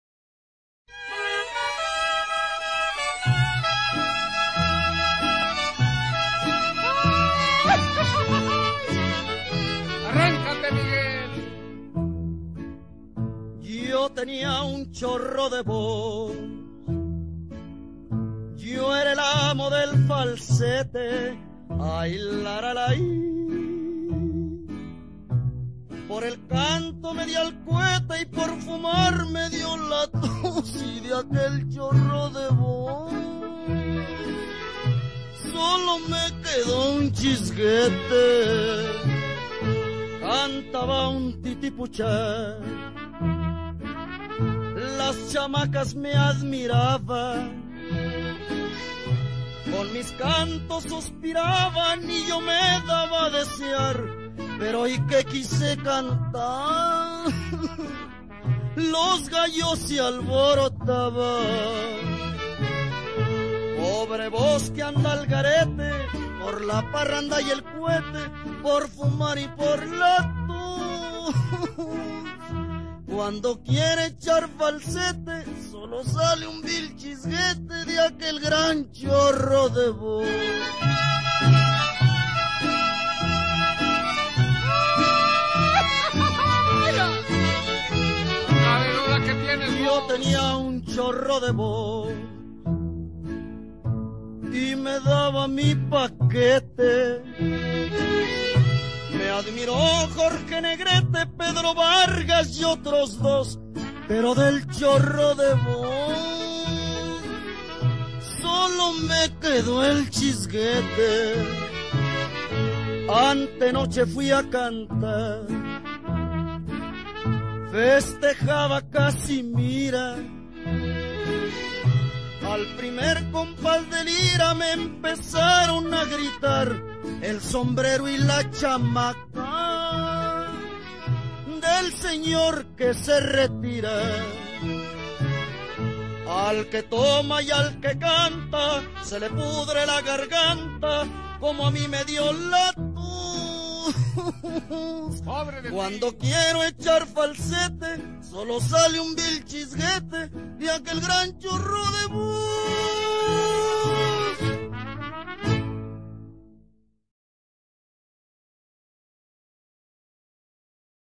El falsete y el sainete
Miguel Aceves Mejía
era el amo del falsete,
y a mí me gustaría
ser el amo del sainete.

Miguel Aceves Mejía
tenía un chorro de voz,
un día se nos moría
y al cielo se la llevó.

Un chorro de sainetes tengo
y vivo estoy todavía,
podré seguir componiendo
este género de la poesía.

Le llaman género menor
pero eso me da igual,
Miguel, no era un tenor
pero cantaba genial.

Que no me llegue a pasar
como a Miguel con el falsete,
que por beber y fumar
se convirtió en chisquete.

En realidad, no sucedía
solo fue en esta canción,
con su falsete seguía
hasta el día que murió.

Miguel Aceves, se nos moría
y aunque el falsete siguió
sin él, lo mismo ya no sería
que él fue quien lo creó:
Miguel Aceves Mejía.

Cuando yo me haya muerto
también el sainete seguirá
y estoy seguro que acierto,
habrá otro que lo escribirá
yo no podré escribir muerto.

Aunque, tanto me llega a gustar
que puede que hasta lo intente,
pero, eso no lo podré contar
que nadie vuelve de la muerte.

También podría pasar
que fuera un privilegiado,
y ser el primero en regresar
después de ser un finado.

Miguel Aceves, no ha vuelto
ni volvieron sus falsetes,
tal vez, cuando yo esté muerto
puedan volver mis sainetes,
si todo eso fuera eso cierto
pondré alguno en un brete
y sembraré el desconcierto.

Le dedico esta poesía
al gran amo del falsete:
Miguel Aceves Mejía.
eso es lo que yo querría.